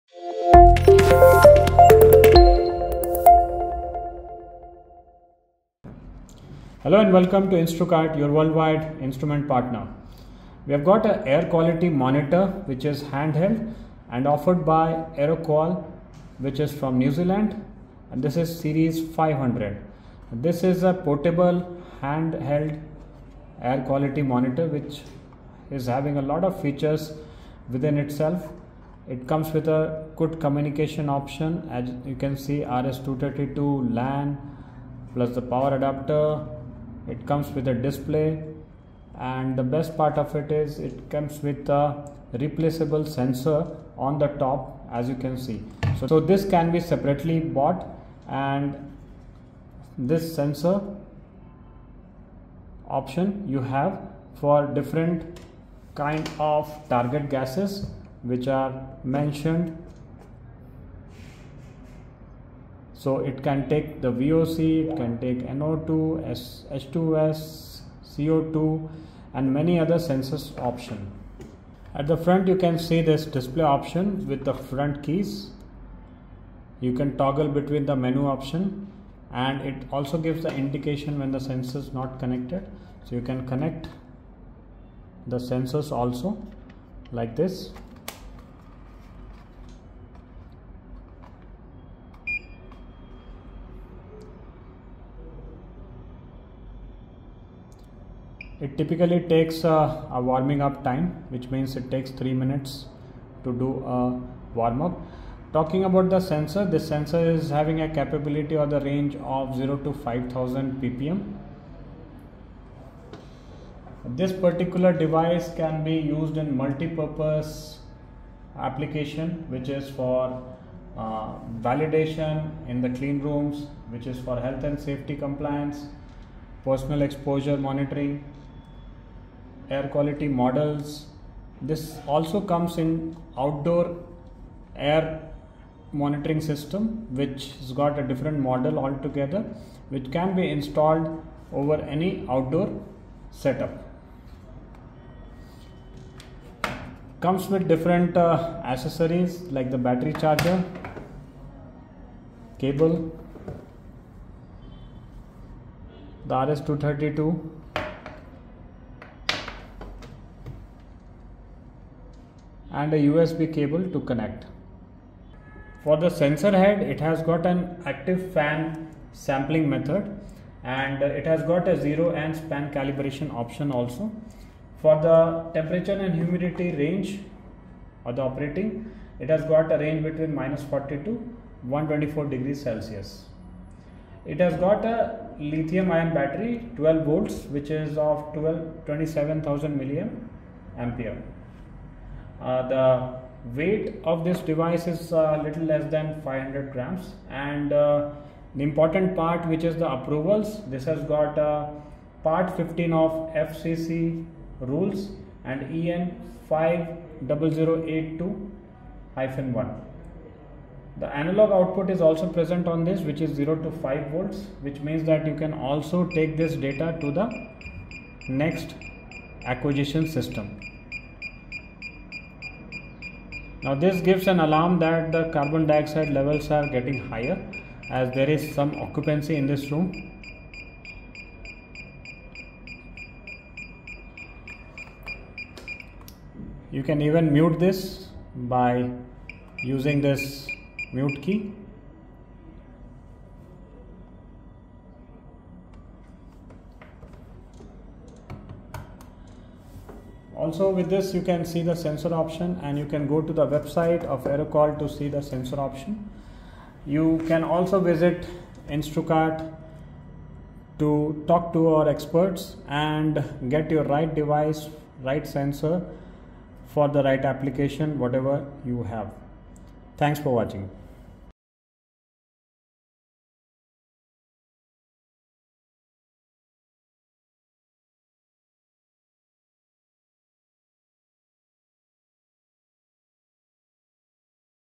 Air Quality Monitor | Aeroqual S500 | Carbon dioxide | Instrukart
Designed for those who need a handheld device to gather real-time
information on the surrounding air. A simple tool for professionals and enthusiasts alike, the portable monitors can be configured with 28 different gas and particle sensors. Suitable for measuring target gases in ambient air
at different concentrations in outdoor or indoor environments.

The Series 500 Portable Indoor Air Quality Monitor is compatible with up to 30 different sensor heads, measuring a range of pollutants (including PM2.5. PM10, CO, CO2, NO2, VOC, O3, SO2, H2S and more.)